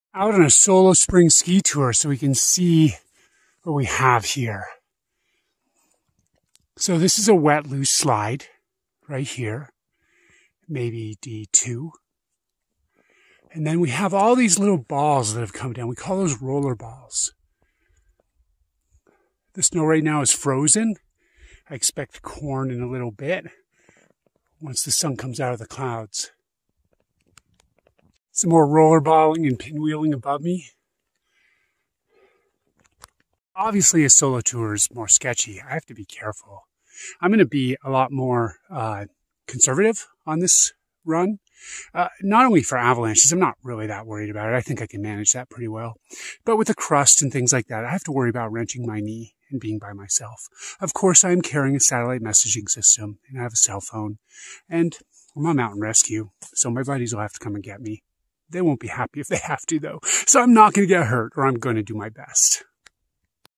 Thoughts on a Solo Ski Tour
Skiing in the backcountry by yourself can be dangerous, especially if you do not have the appropriate experience and training. This video takes you into the mind of an individual on a solo ski tour..

The American Alpine Institute runs a wide array of backcountry skiing, splitboarding, snowshoeing and avalanche rescue programs.